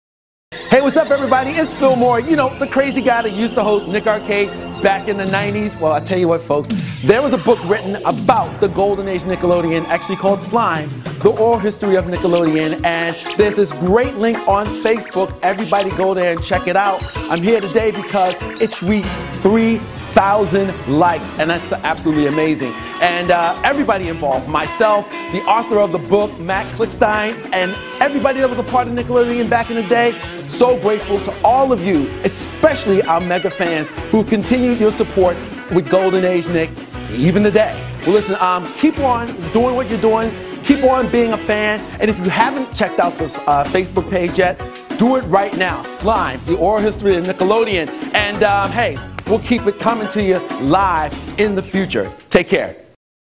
Phil Moore: Nick Arcade 2014
At the 2013 Nickelodeon event in NYC, Phil Moore expressed his interest to fans in a Nick Arcade reboot.

Check out the new Nickelodeon 80s/90s book SLIMED by Matthew Klickstein